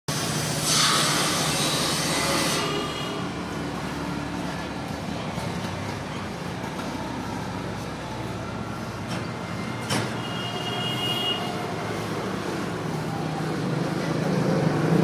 Wooden Dining Chair, Bed & Sofa Deco Polish & Cusion Work progressing
manufecturer & suppliers of wooden handicrafts & all kind of timber trader
fiveseven sofa
natural polish